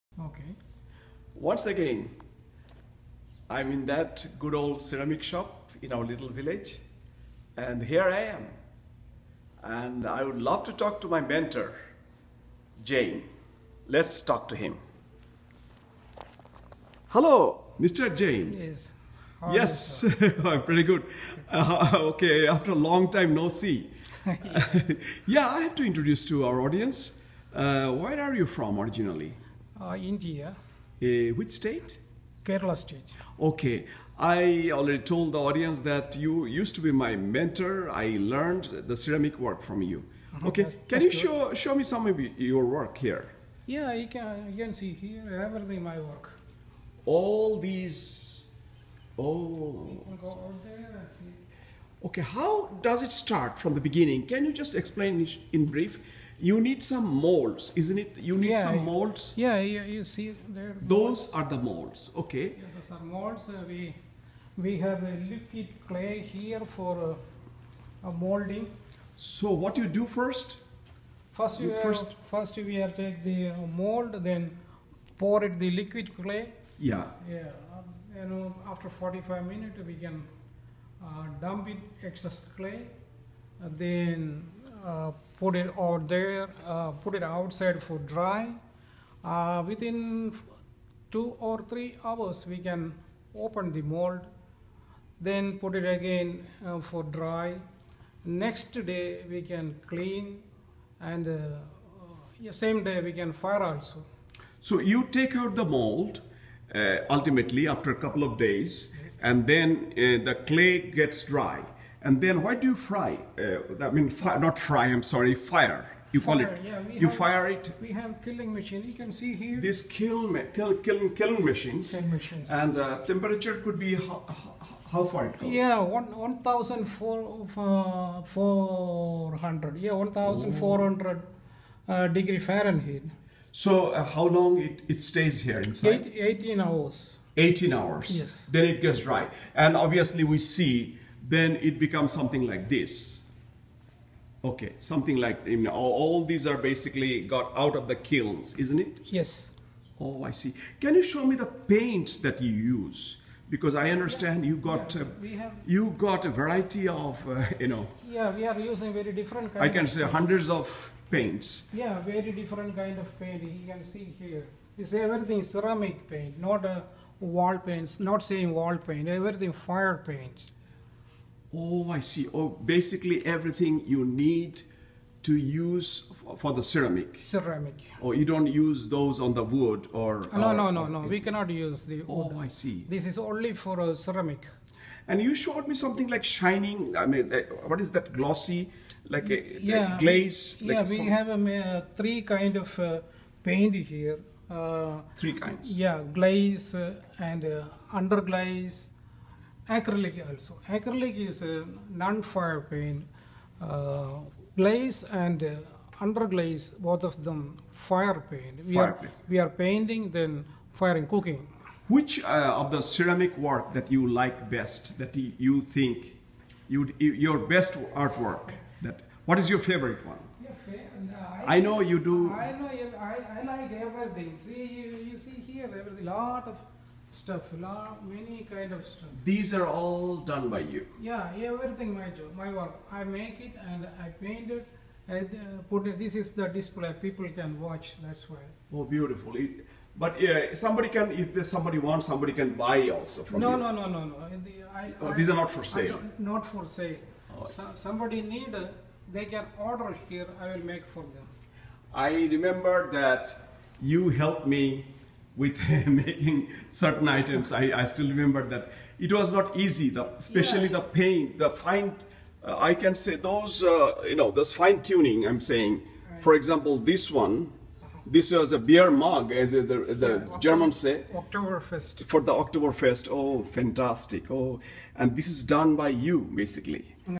Interview of Jain, a Ceramic artist
This evening I dropped by my favorite Ceramic Shop. Here is my conversation with my mentor Jain, who taught me the ABC’s of ceramic art.